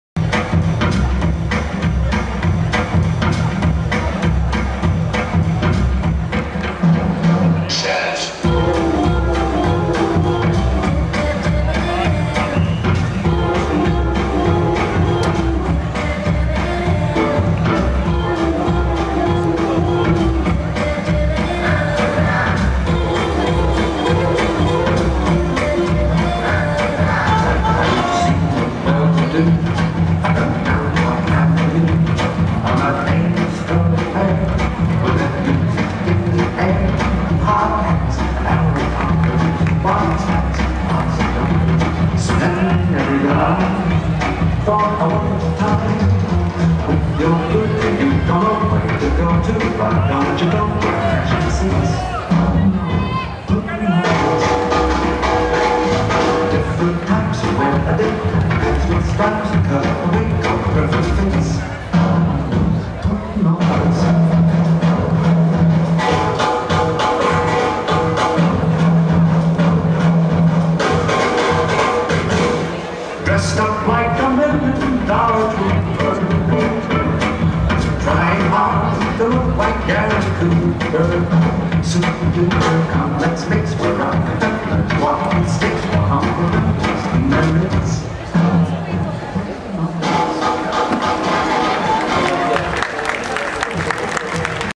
Pavel Mareš, Michaela Ročejdlová - Quickstep
Záznam z taneční soutěže mezinárodní třídy "M" a rychlý foxtrot QUickstep.